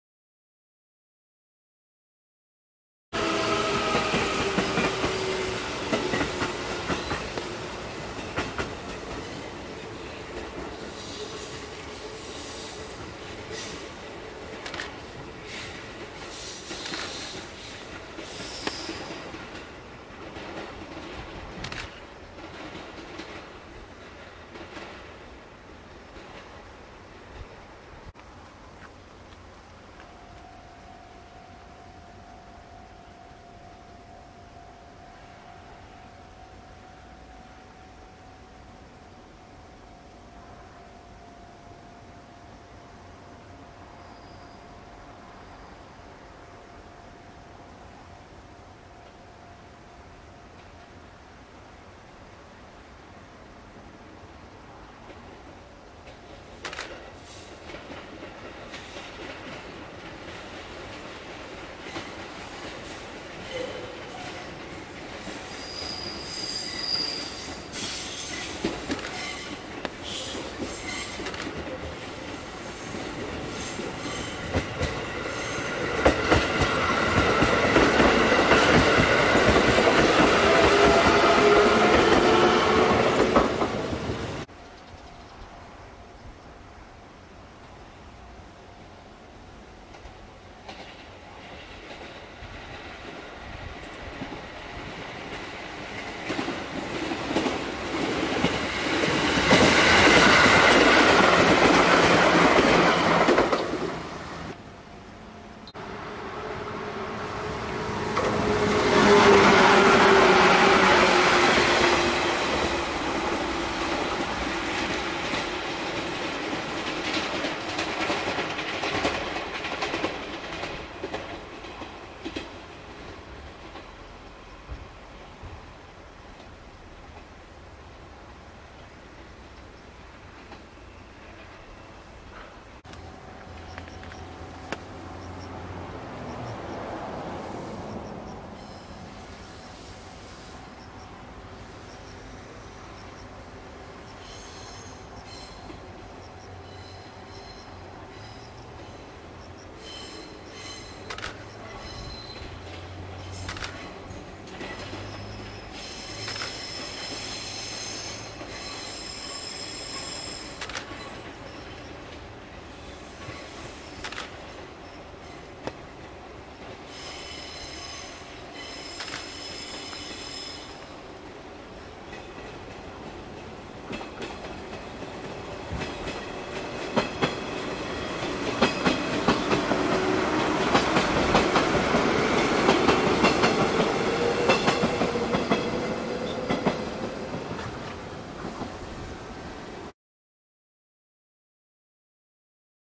東海道本線 富士駅 懐かしい列車 115系 167系など
富士駅を発着する湘南色の115系、東海道本線から身延線に乗り入れる167系団体列車など、懐かしい列車の動画をお楽しみ下さい。